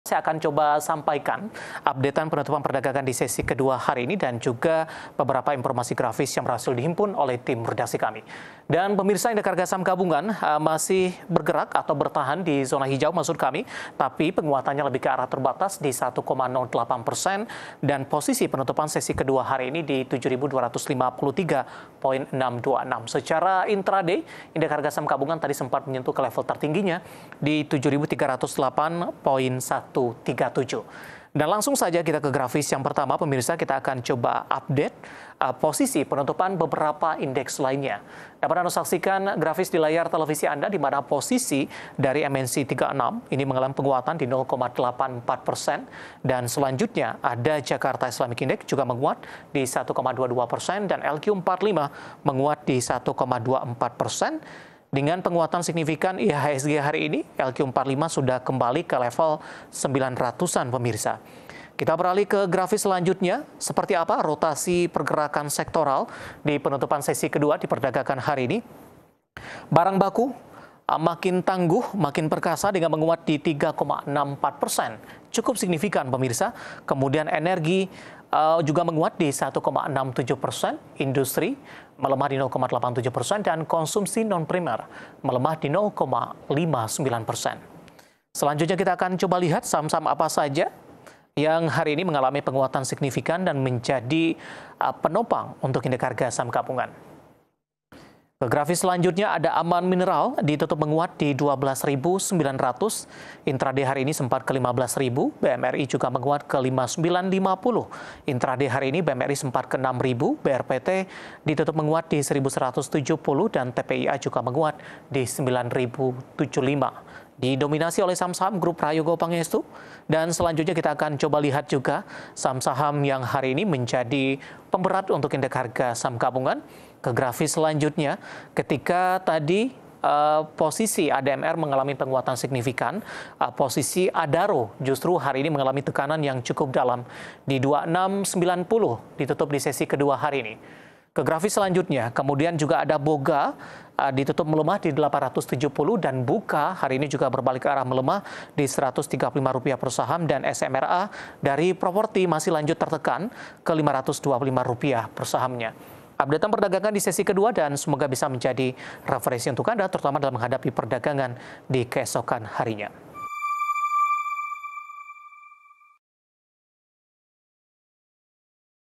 IHSG Sesi II Menguat ke Level 7.253 | IDX CHANNEL
Indeks Harga Saham Gabungan (IHSG) ditutup menguat 77,20 poin atau 1,08% ke level 7.253,62 pada Selasa (28/5/2024).

Terdapat 287 saham menguat, 240 saham melemah dan 253 saham stagnan. Transaksi perdagangan mencapai Rp13,4 triliun dari 19,5 miliar saham yang diperdagangkan.